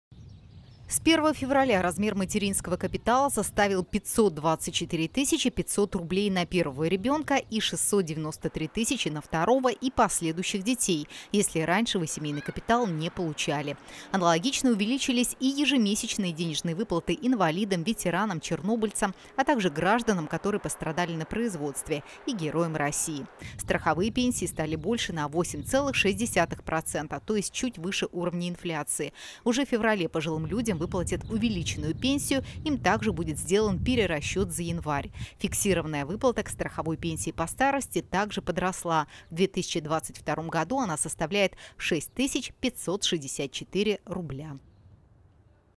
Социальные пособия с 1 февраля подросли
Социальные пособия, а также материнский капитал с 1 февраля стали больше. Индексация составила 8,4 % - таков официально средний уровень инфляции по стране за прошлый год,

С 1 февраля размер материнского капитала составил 524 тысячи 500  рублей на первого ребенка и 693 тысячи  на второго и последующих детей, если раньше вы семейный капитал не получали. Аналогично увеличились и ежемесячные денежные выплаты  инвалидам, ветеранам, чернобыльцам, а также гражданам, которые пострадали на производстве, и Героям России.

Страховые пенсии увеличились на 8,6%, то есть чуть выше уровня инфляции. Уже в феврале пожилым людям выплатят увеличенную пенсию, им также будет сделан перерасчет за январь. Фиксированная выплата к страховой пенсии по старости также подросла. В 2022 году она составляет 6 564 рубля.